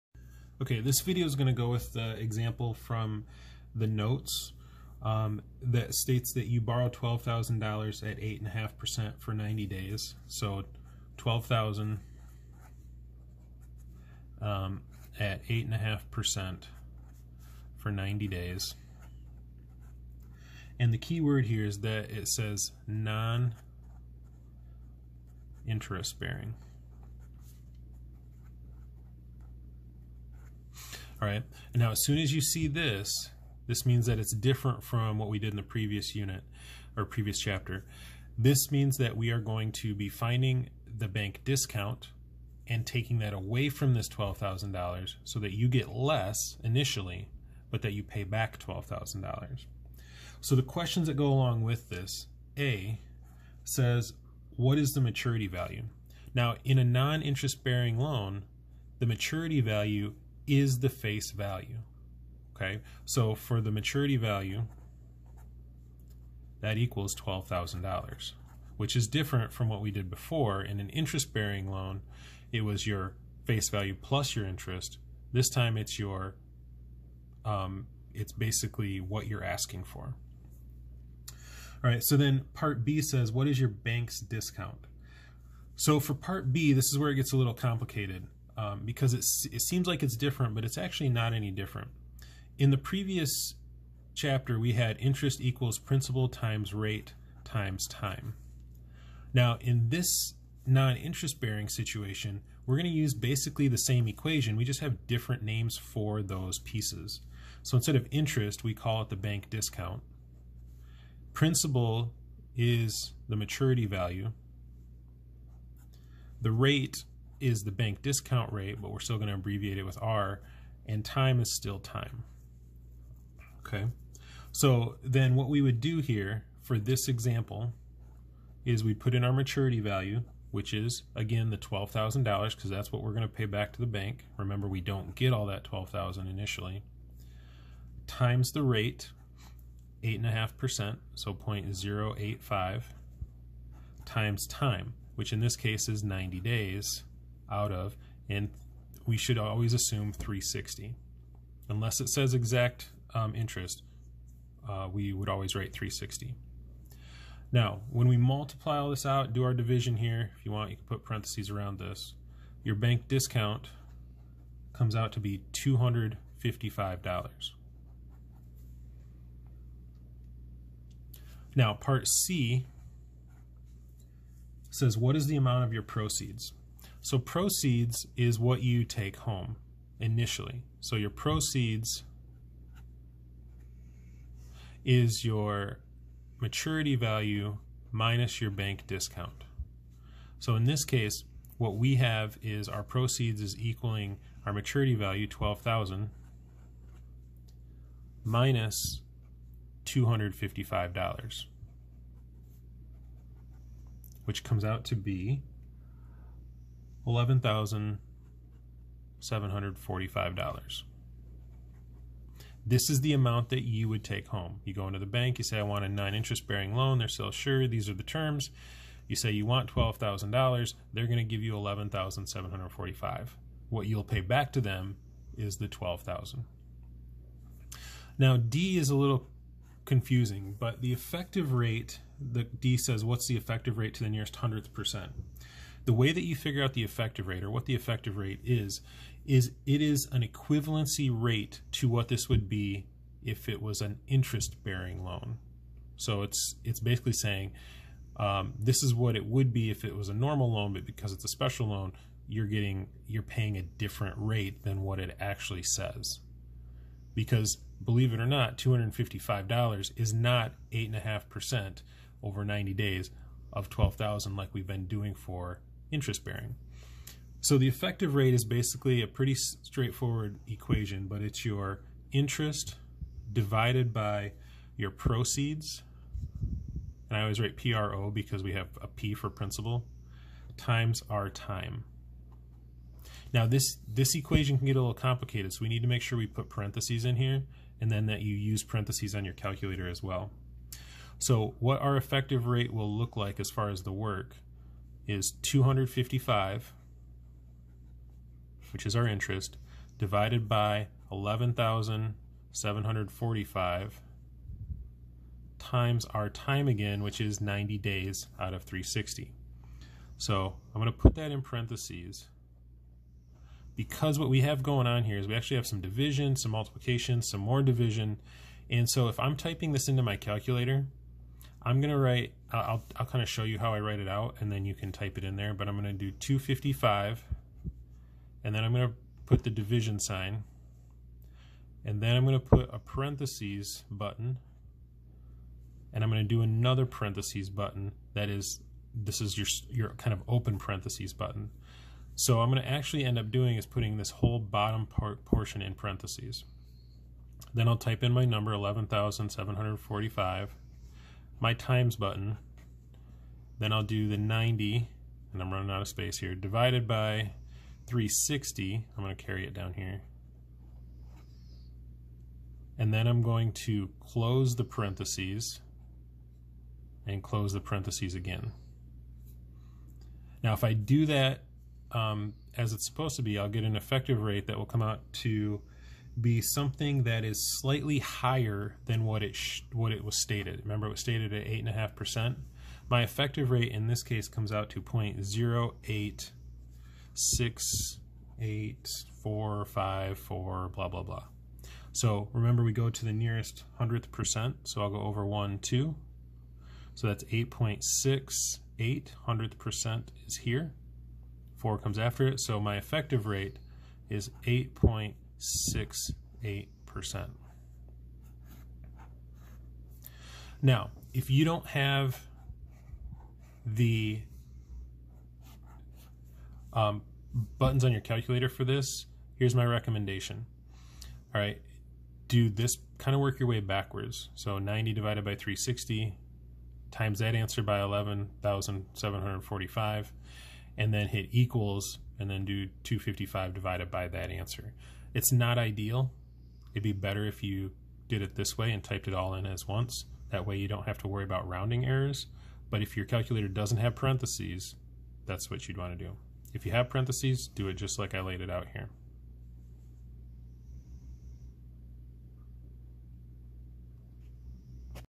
Simple Discount Note Example 1
This goes along with slide 7 of the notes.
You borrow $12,000 at 8.5% for 90 days on a non-interest-bearing, simple discount note. (a) What is the maturity value? (b) What is the bank’s discount? (c) What is the amount of your proceeds? (d) What is the effective rate to the nearest hundredth percent?